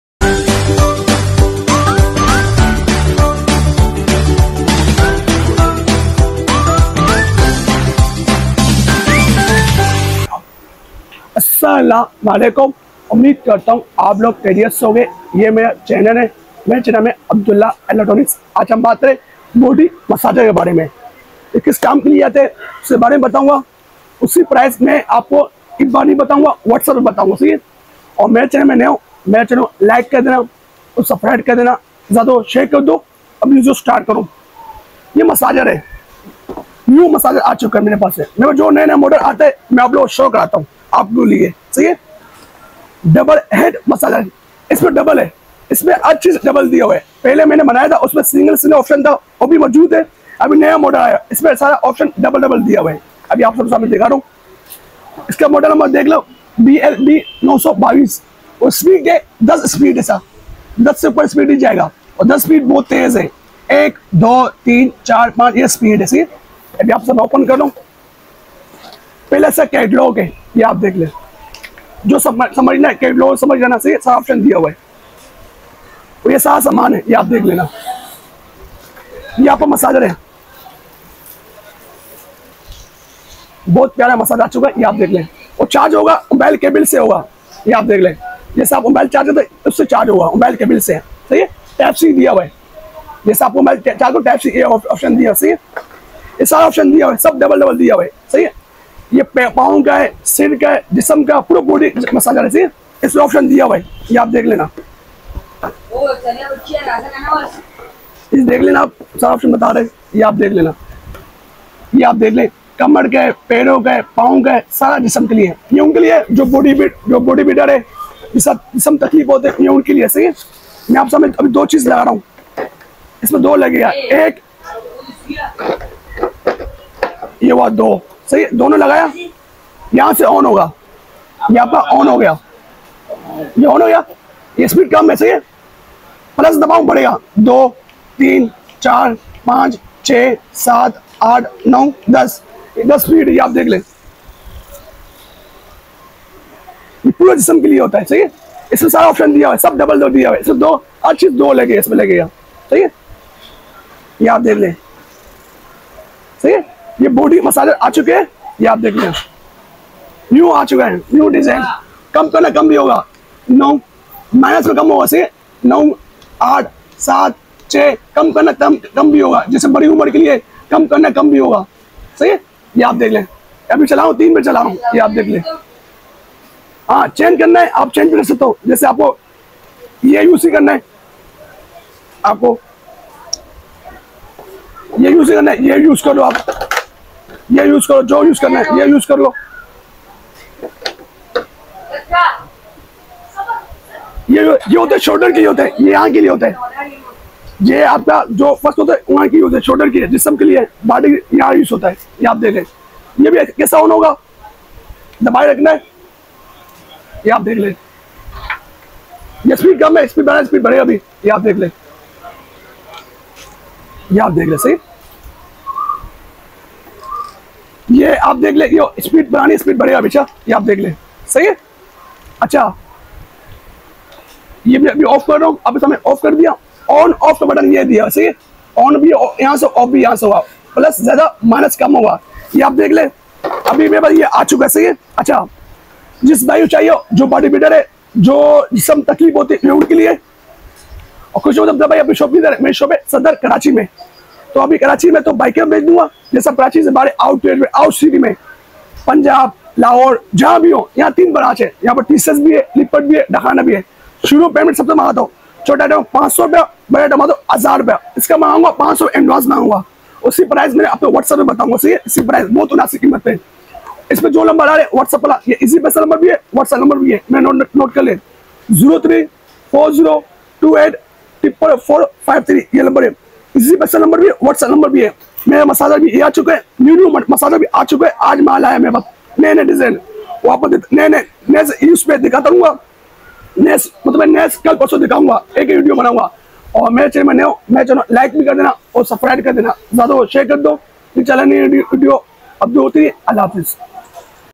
Unboxing Double-Head Massager 10 Speed level Fascial Gun BlD-922 || Best body Massage || Urdu/hindi
Hakeem center sadder Karachi shop number 17